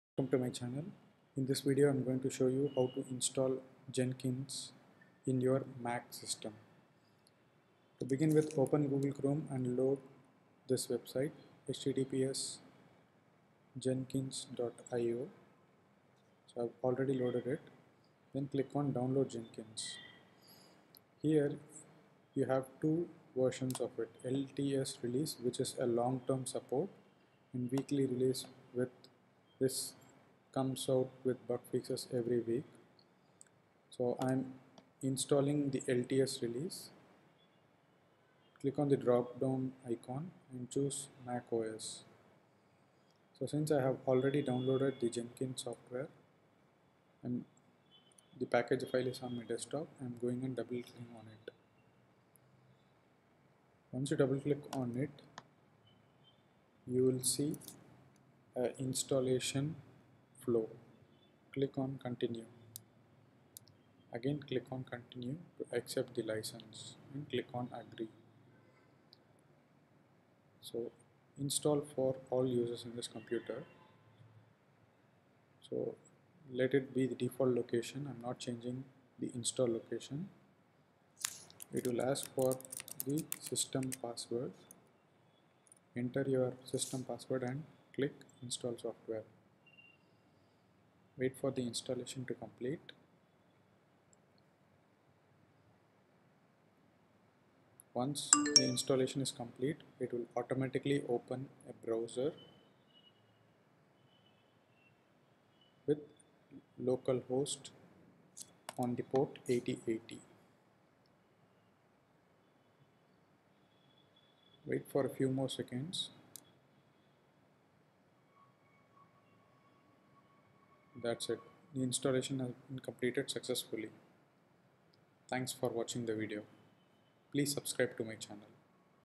How to Install Jenkins in Mac - Simple steps to install Jenkins
This video shows the steps to install Jenkins in your Mac. Jenkins is a very important tool in software development and software testing stages.

Jenkins is the leading open source automation server, Jenkins provides hundreds of plugins to support building, deploying and automating any project.